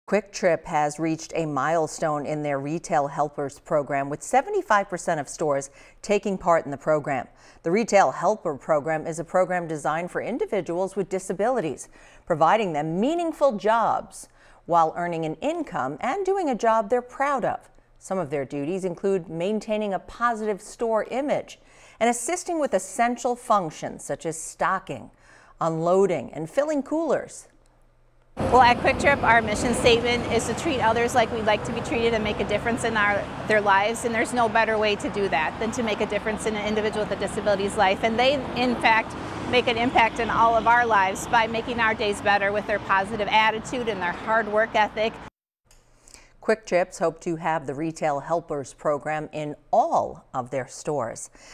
Retail Helpers program now in 75% of Kwik Trip stores
Kwik Trip has reached a milestone with 75% of stores employing retail helpers.